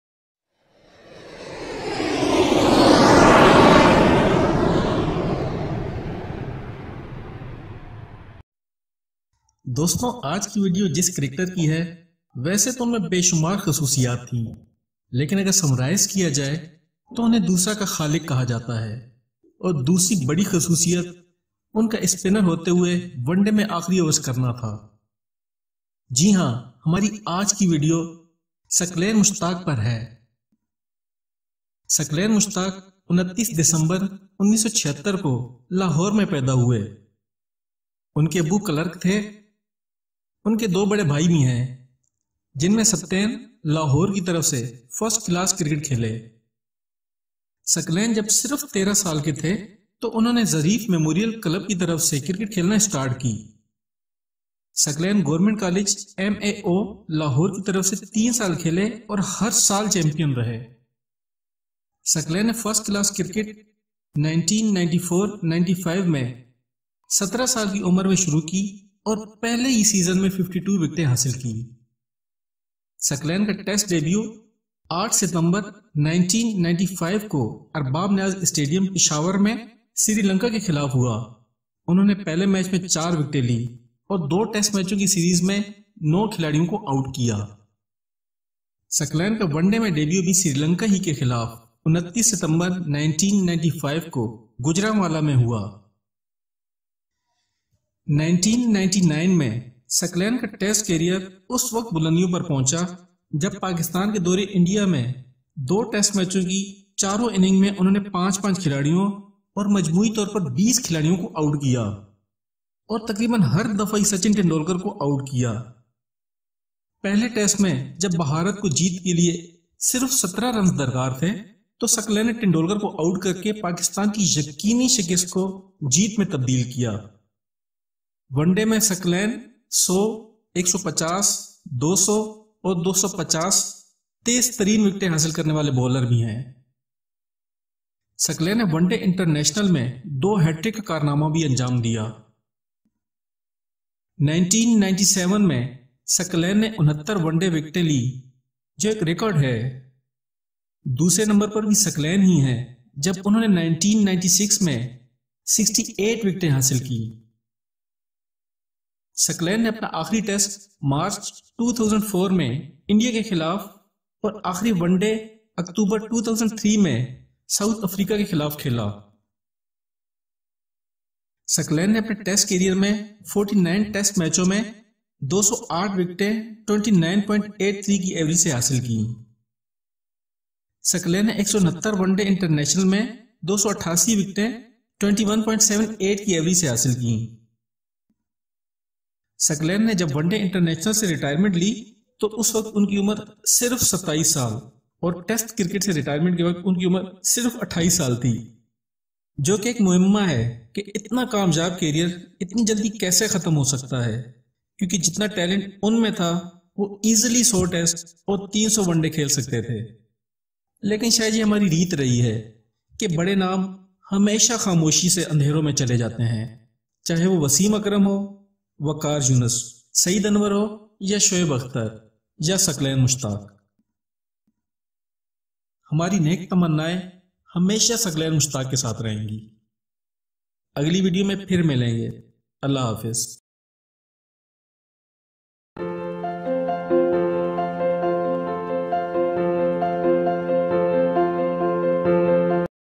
Saqlain Mushtaq Life Story | Biography | Early Life | Career | with English Subtitles
Saqlain Mushtaq is a former cricketer from Pakistan, born in Lahore on 29 December 1976. He was a right arm off-break bowler and is one of the best spinners
cricket has ever seen.
He has a mean spin control and is renowned for his ‘doosras’. In fact, he was one of the first spinners to have discovered this method of bowling where the
batsman is tricked into feeling that the ball will spin one way, but spins the other way.
Saqlain was not good enough with the bat, but his bowling abilities gave him a place in the national side for over nine years.
Background
Being born to a clerk in Lahore, Mushtaq and his brother Sibtain (who played first-class cricket for Lahore) had a hard time seeking development through academies.
Saqlain started playing cricket at the Zareef Memorial Cricket Club Second Eleven at an age of just 13.
He had declared that he wanted to be an off-spinner ever since he began playing the sport. Mushtaq went on to play for the Government M.A.O College, Lahore and
won the cricket championship for three successive years.
Saqlain has represented the following teams - Ireland, Islamabad Cricket Association, Lahore Badshahs, Pakistan International Airlines, Old Whitgiftians CC, Surrey,
and Sussex.
Debut
Saqlain Mushtaq made his international debut on 8 September 1995 in a test against Sri Lanka. He picked up two wickets in the first innings and two more in the
follow-up where Pakistan beat Sri Lanka by an innings and 40 runs. He had a total of nine wickets in the series.
On 29 September 1995, Saqlain made his ODI debut against Sri Lanka at Gujranwala. He ended up wicketless in a nine wicket win for Pakistan.
Rise to Glory
Saqlain Mushtaq’s career was a glittering one for the time he played. At his time, he broke the world record to become the quickest to reach 100 ODI wickets
(which was broken by Australian pacer Mitchell Starc).
His big break came in the next test series against Sri Lanka where he picked up a remarkable 14 wickets in just two tests. A string of top performances followed,
including a nine-wicket haul against the West Indies and a five-wicket haul against Zimbabwe.
The highlight of Mushtaq’s career came in 1999, in a test series against India. He picked up his first 10-wicket haul in the first test which included a second
innings thriller, where Sachin Tendulkar fell for 136 against Mushtaq. In the second test, he once again picked up 10 wickets, but could not help his team to a win.
Saqlain’s test career continued on the same path as he picked up 10 wickets in 2 tests against Australia in 1999, and 8 wickets against England in 2000.
He has two hat-tricks to his name, both coming against minnows Zimbabwe. He is also the first Pakistani player to claim a hat-trick in a World Cup.
Domestic Career
Saqlain’s domestic career began at Pakistan International Airlines at the age of 17. He picked up 52 wickets in the first season which effected a selection
for the Pakistan A team. Saqlain then moved on to Surrey, where he played eight consecutive seasons.
In 2007, he signed for Sussex in a two-year deal and then returned to Surrey in the same year. He was released by Surrey in 2008, which effectively ended his
domestic career.
Saqlain has 833 first-class wickets from 194 matches, and 478 List-A wickets in 323 matches.
Retirement
Saqlain Mushtaq retired from international cricket around 2004-05 due to constant injuries and the emergence of other spinners for the national team. He played
his last test in 2004 and his last ODI in 2003.
In domestic cricket, he played his last game for Surrey in 2008 and ended his cricketing career in the same year.

sportscrazecentre